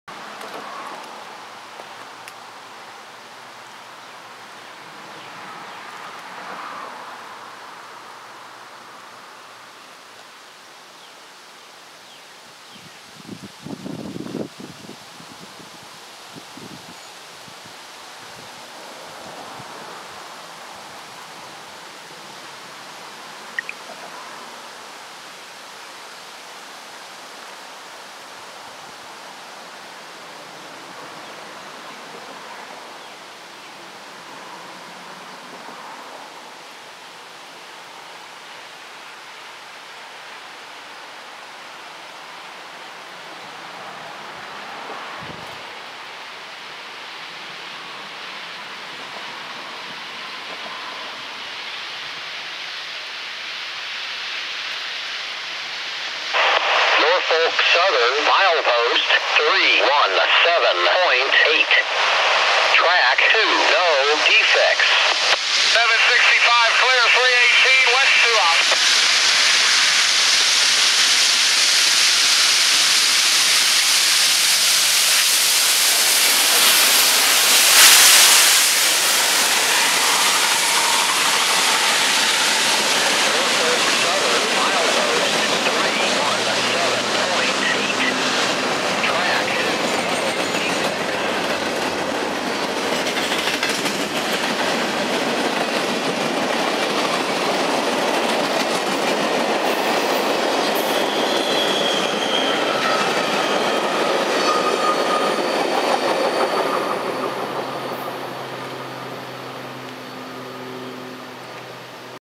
NKP 765 at George's Station
NS 061 knocks down the signal at George's Station as it makes its way to Conway.

NS 061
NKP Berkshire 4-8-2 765
NS ES44AC 8102 "PRR"
NS ES44AC 8098 "CR"